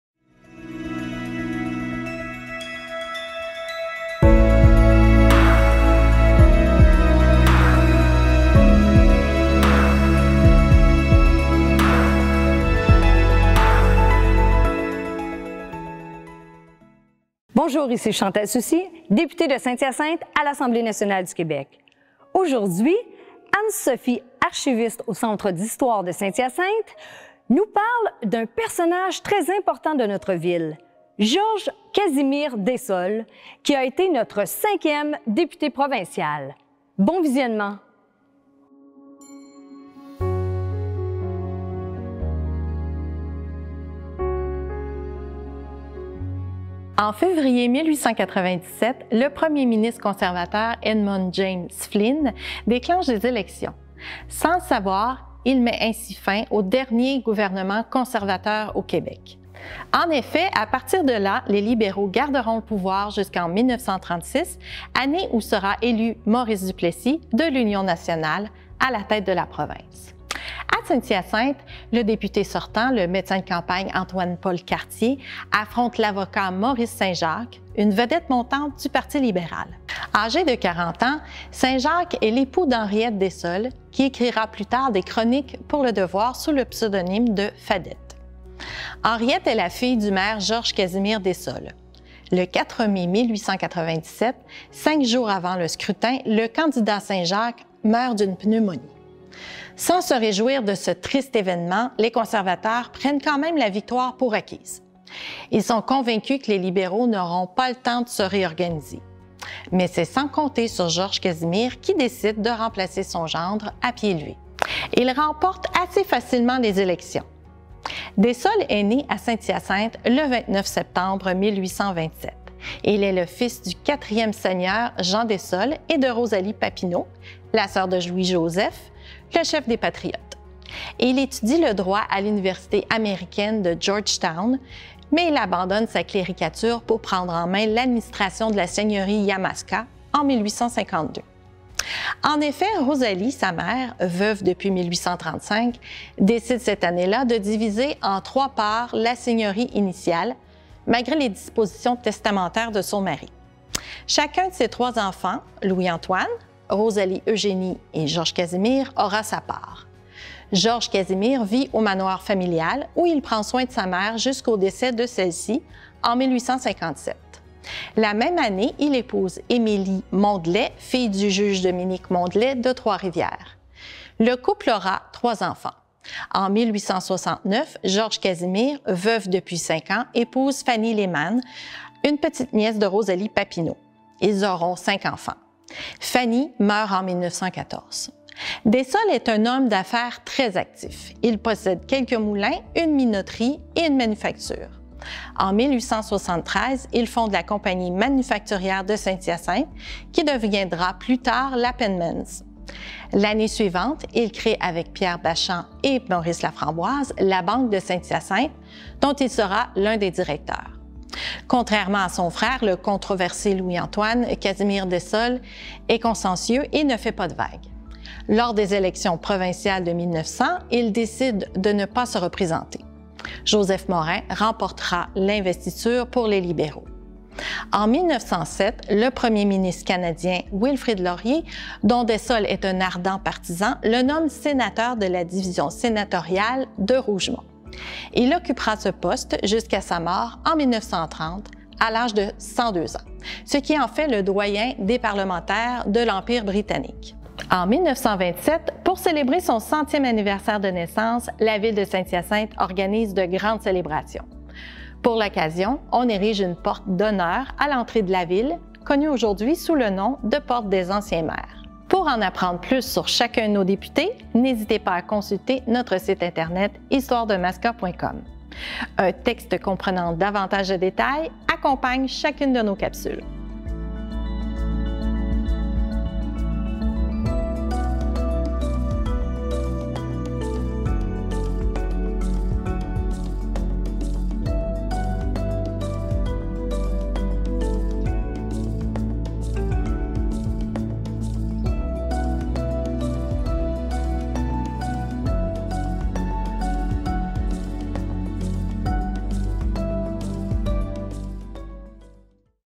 Nos élus à Québec. Clip 6 Georges-Casimir Dessaulles
Cette série de capsules, présentée par Madame Chantal Soucy, députée de Saint-Hyacinthe à l'Assemblée nationale, dresse le portrait des représentants du comté à Québec. Une vidéo, accompagnée d'un texte, nous parle du parcours de chacun de nos élus à partir de la Confédération.

Pour lire le texte complet, visitez la page « Nos élus à Québec: brève histoire des députés de Saint-Hyacinthe », publiée sur le site du Centre d'histoire de Saint-Hyacinthe.